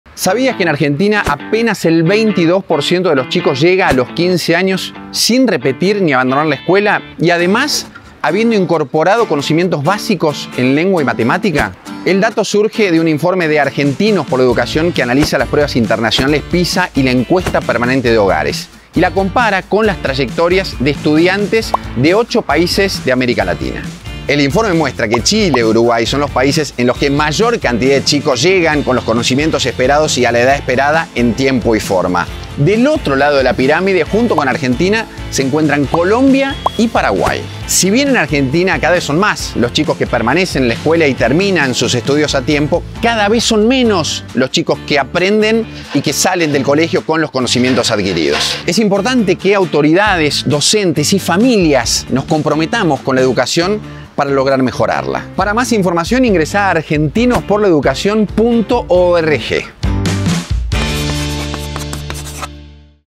Ranking regional: ¿Dónde está parada Argentina en materia de educación?
🏆Palpitando la final de una nueva Copa América armamos un ranking regional para entender cómo se comparan los estudiantes argentinos de 15 años con sus pares de otros países de Latinoamérica.

🥇¿Llegaremos al podio? Escuchá a Federico Seeber que te lo cuenta.